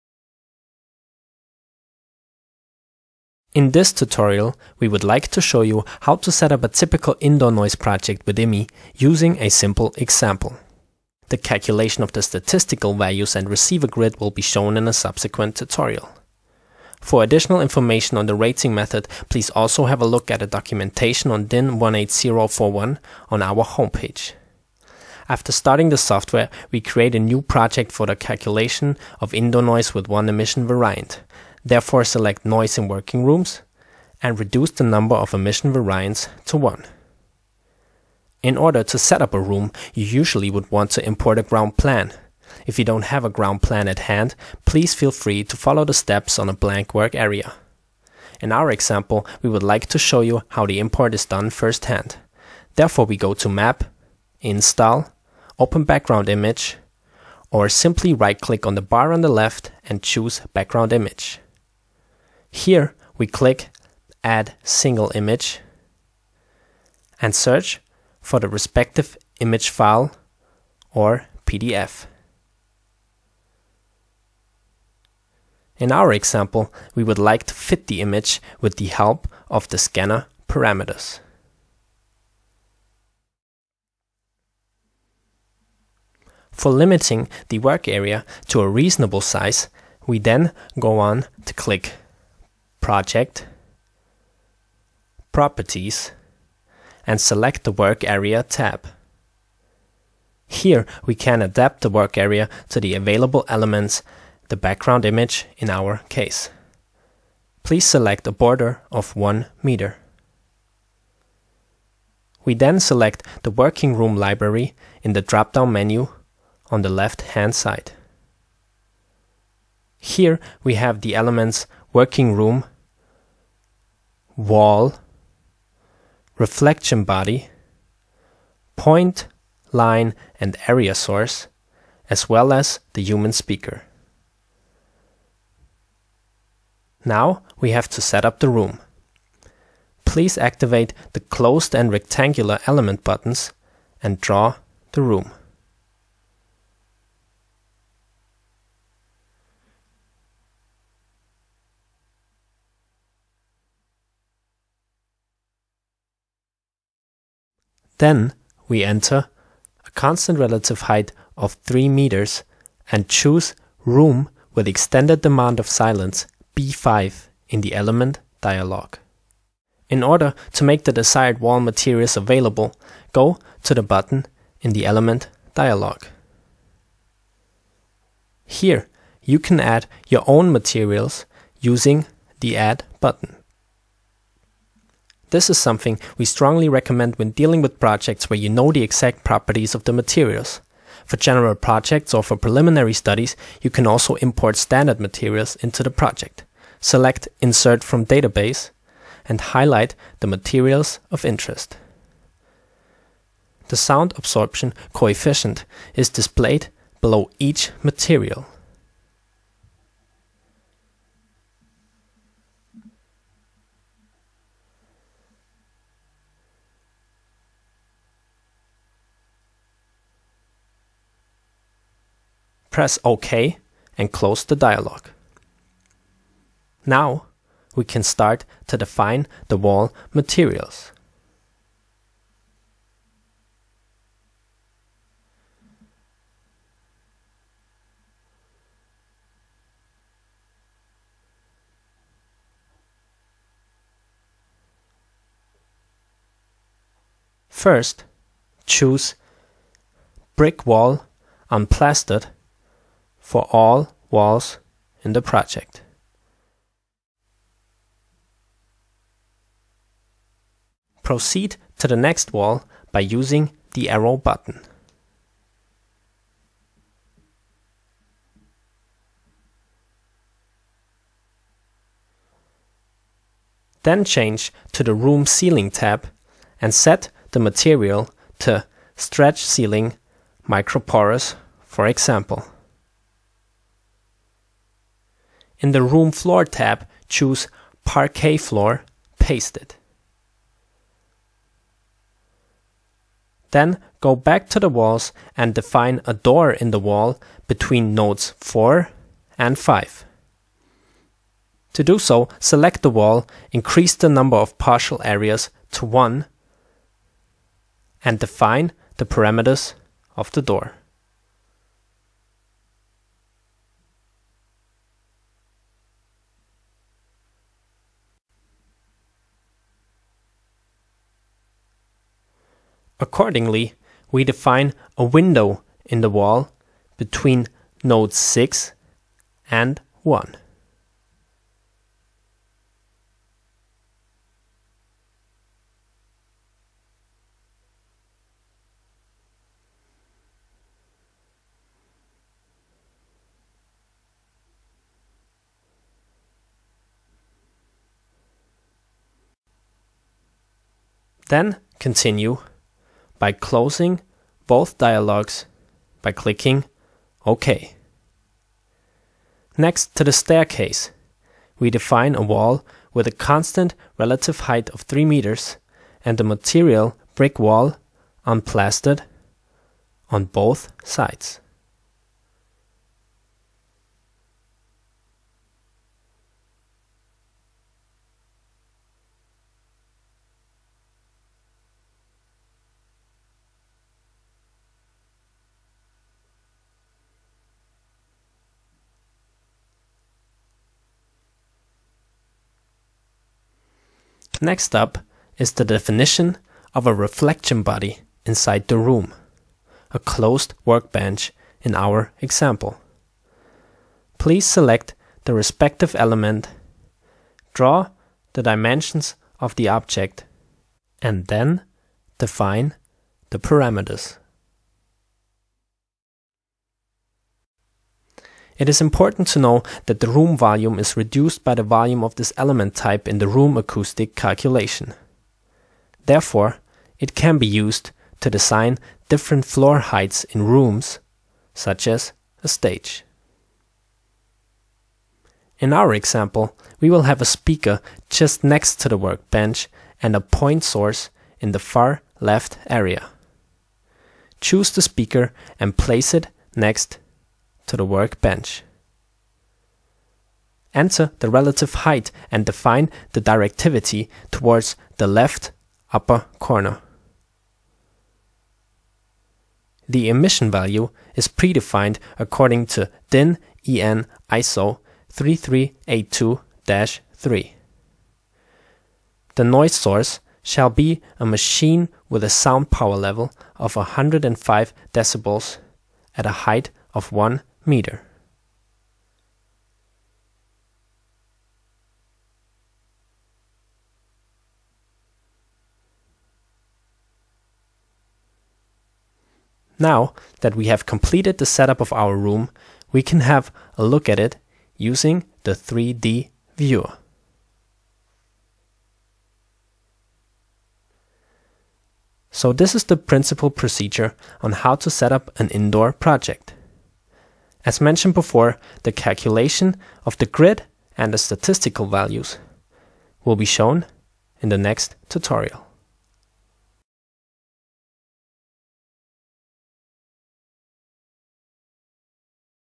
IMMI | Tutorial: indoor module part 1
How to setup a typical noise indoor project in IMMI, the immission control software.